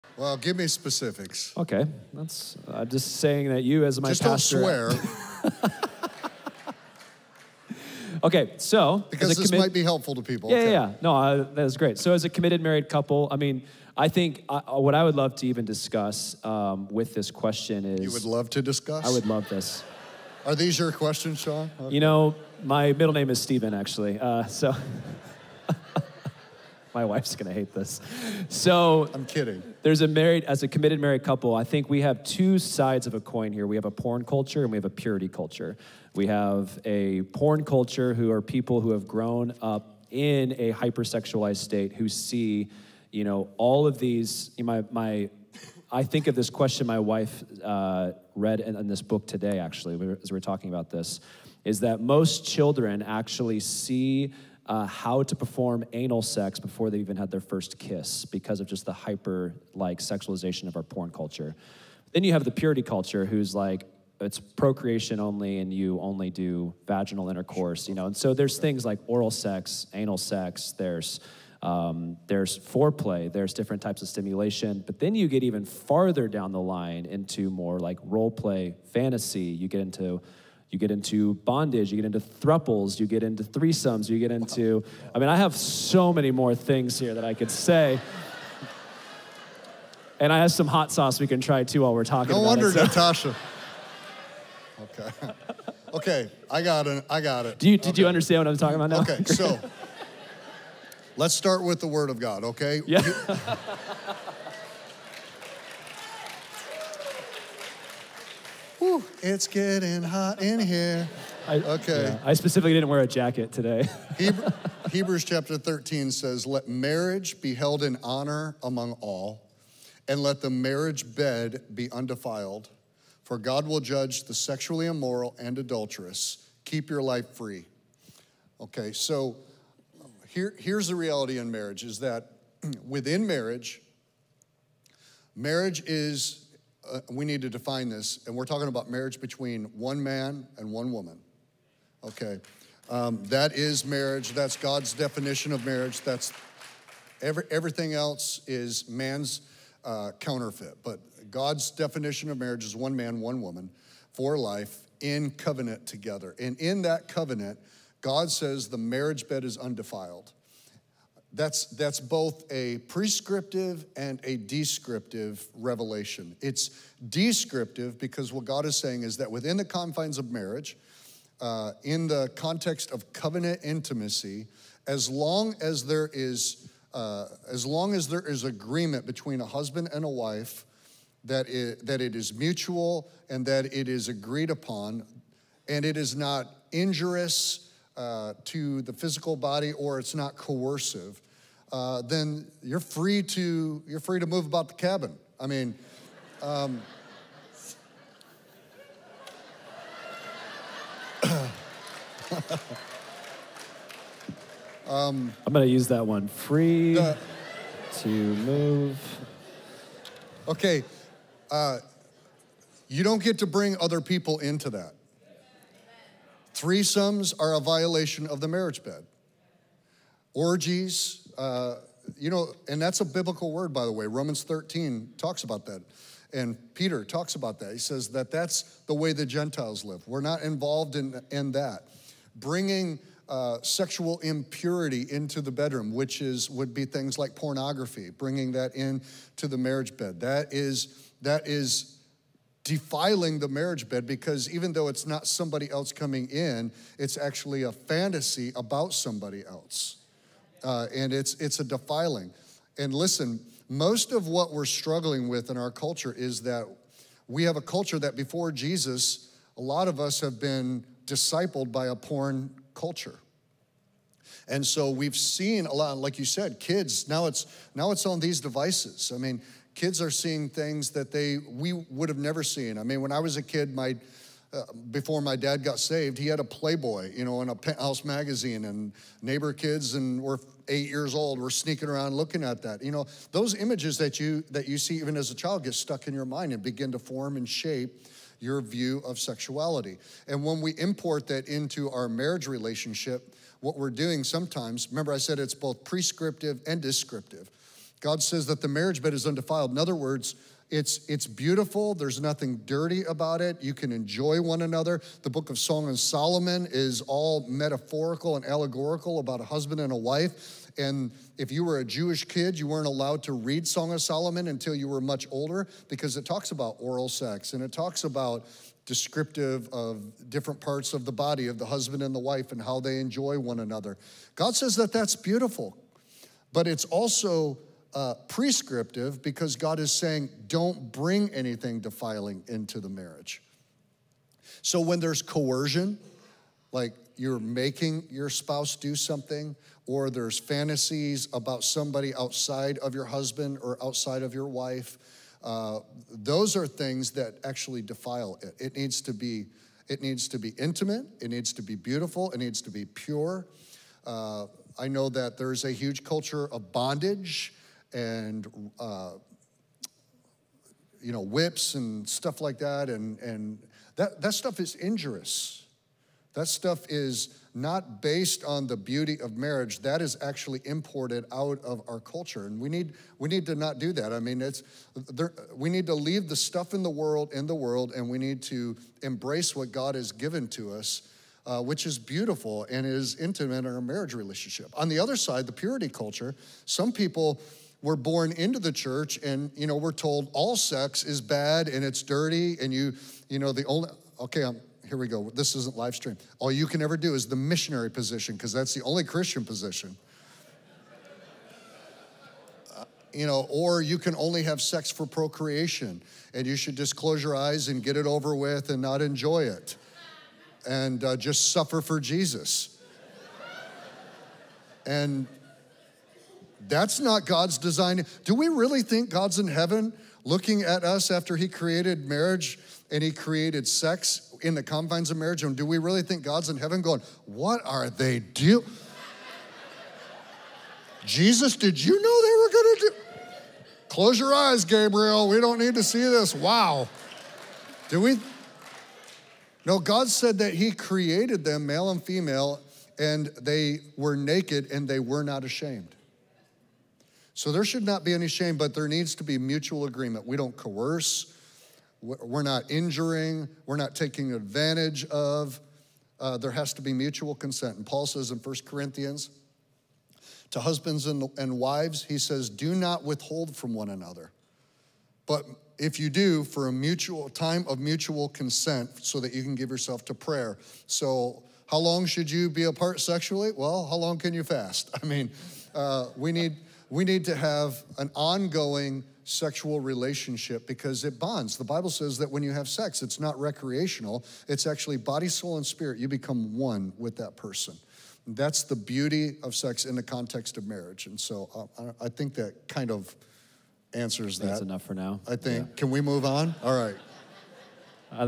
Are Any Sexual Acts or Practices Sinful in Marriage? | Red Hot
This video is from Radiant Church's RED HOT series, an unscripted time with Pastor Lee Cummings where you can ask your burning questions and get immediate, unprepared responses.

The Bible affirms that the marriage bed is undefiled when intimacy is mutual, consensual, non-injurious, and non-coercive, serving as both a prescriptive and descriptive model of covenant intimacy between one man and one woman.

1 Corinthians emphasizes the importance of mutual consent in marital intimacy, advising spouses not to withhold from each other except for agreed-upon periods of prayer.

Modern society presents two problematic extremes: a hyper-sexualized porn culture exposing children to explicit content prematurely, and a purity culture restricting intimacy to procreation-only intercourse.

Practices like bondage, threesomes, and orgies are considered violations of the marriage bed, while coercive fantasies and pornography are deemed to defile it.

The Bible describes marital sex as a union of body, soul, and spirit, with the Song of Solomon offering a metaphorical and allegorical depiction of spousal enjoyment that God considers prescriptive and beautiful.